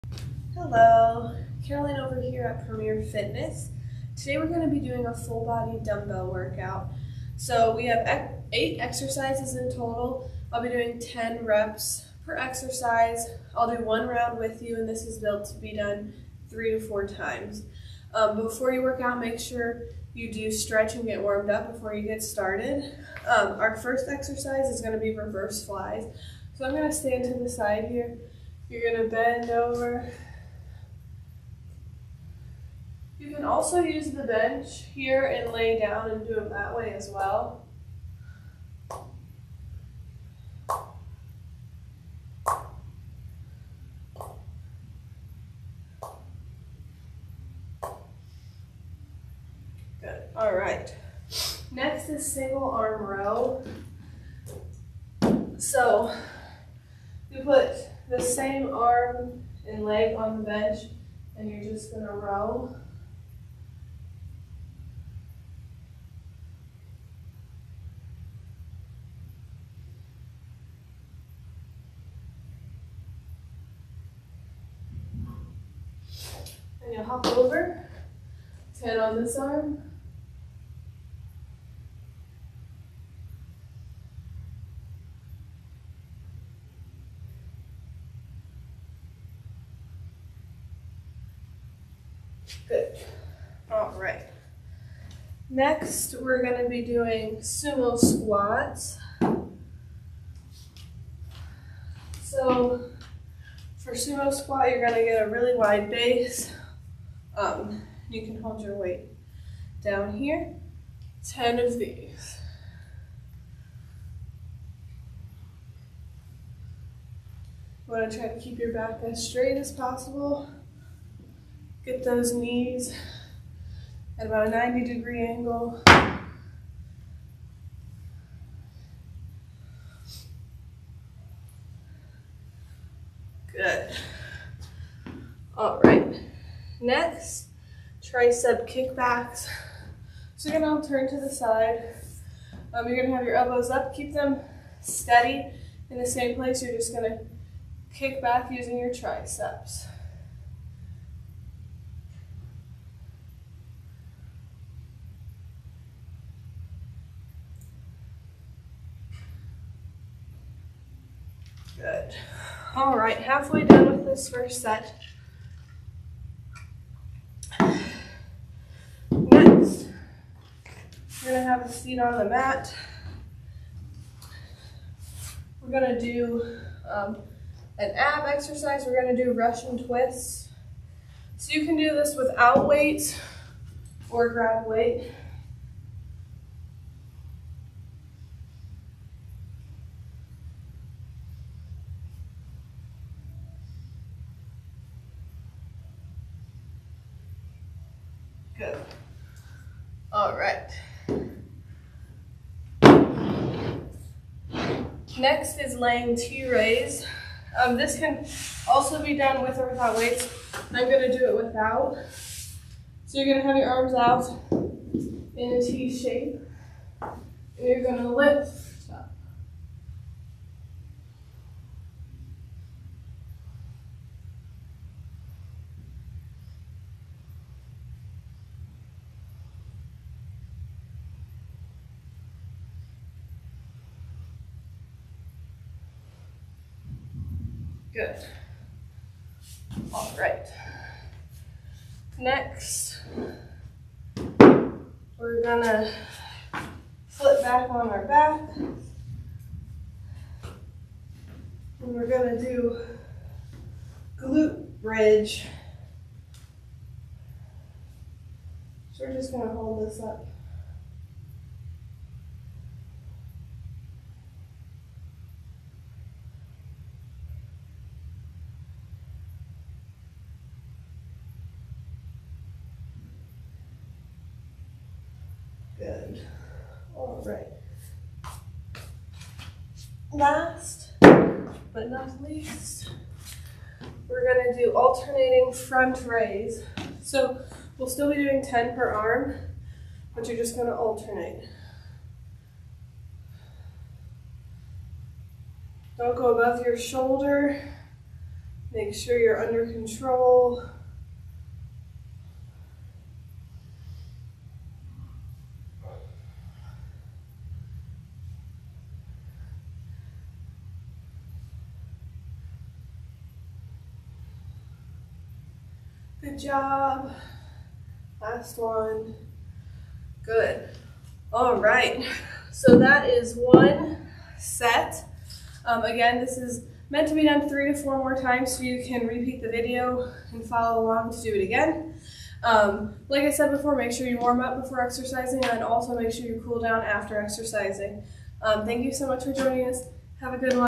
ACMC Premiere Fitness-Full Body Workout with Caroline
This workout consists of 8 exercises. Each exercise is done 10 times per set, for a total of 3 rounds. Be sure to warm up and stretch before exercising and take time to cool down afterwards!